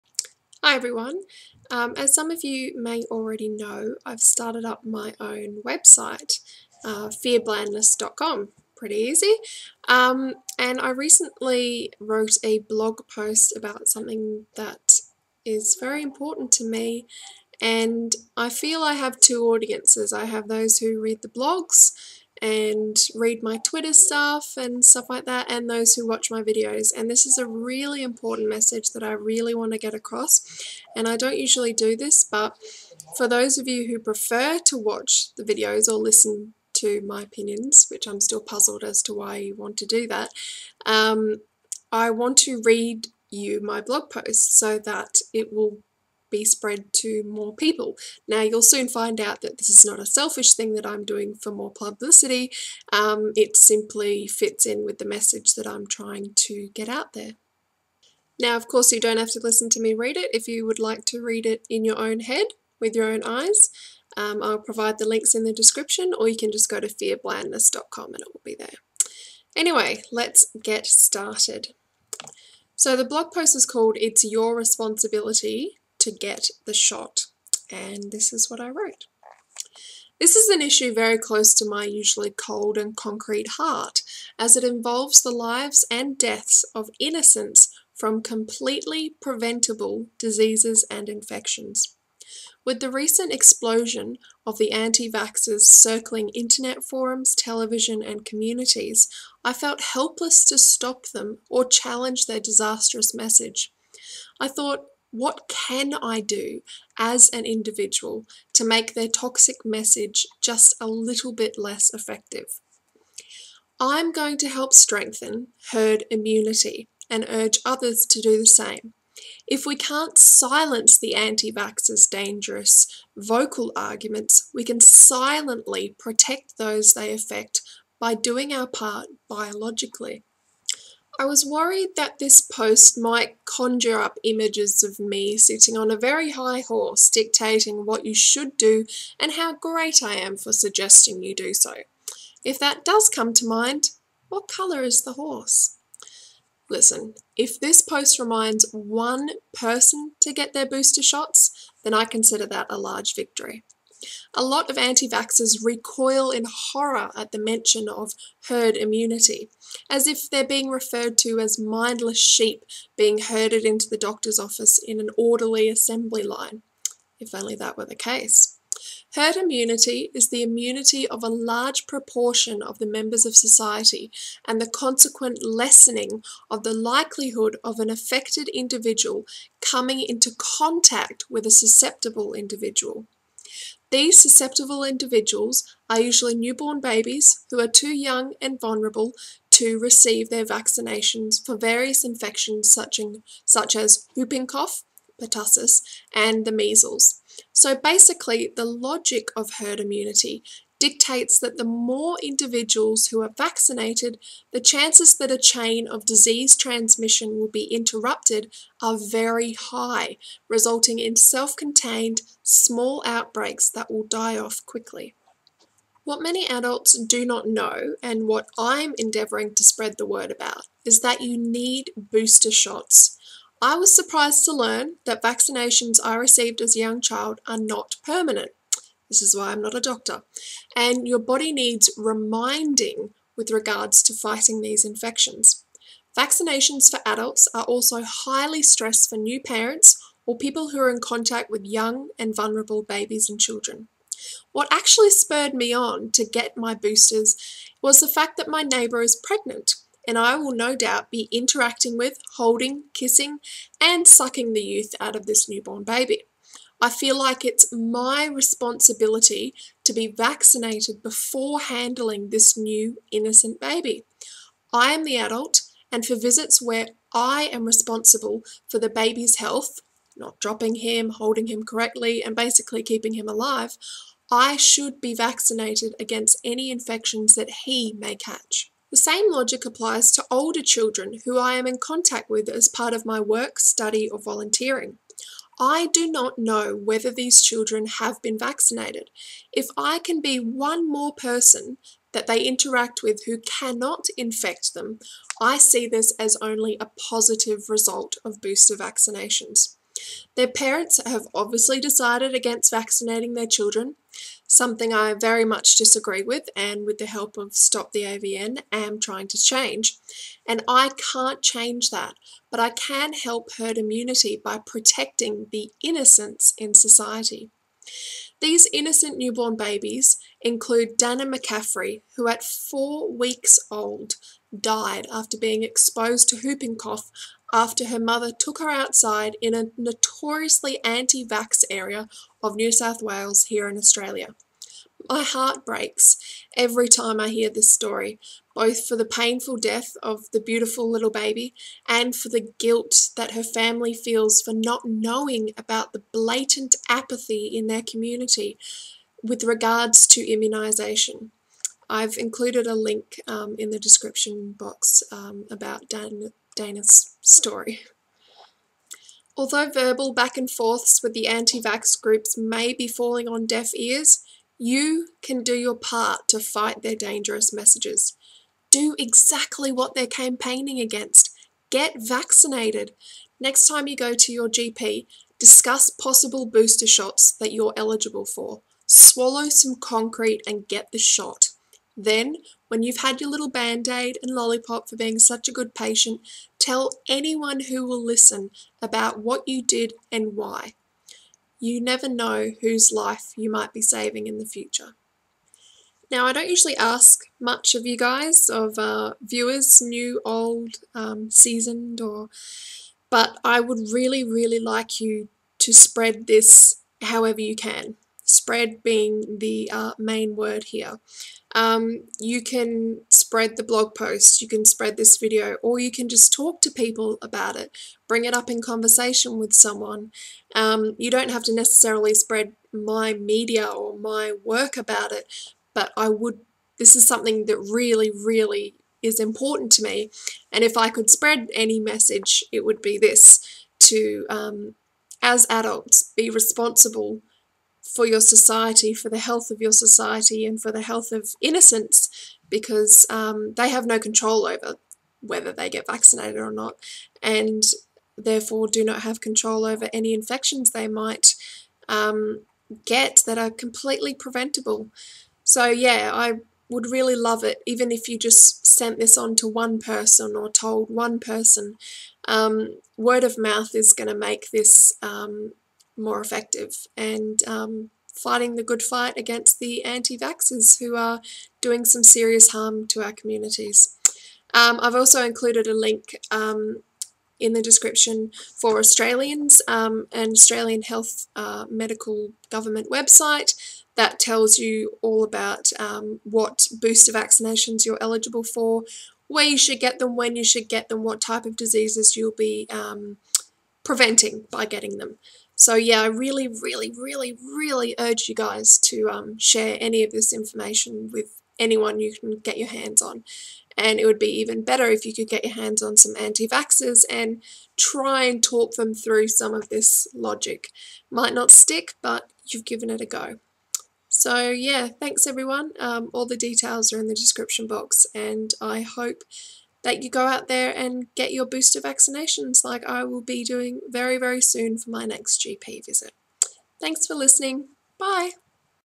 VERY IMPORTANT: Get Your Shots
I stress the importance of booster vaccinations and the idea of herd immunity. If we can't make the anti-vaxers listen, we can fight them biologically.
Tell anyone who will listen. Spread the word!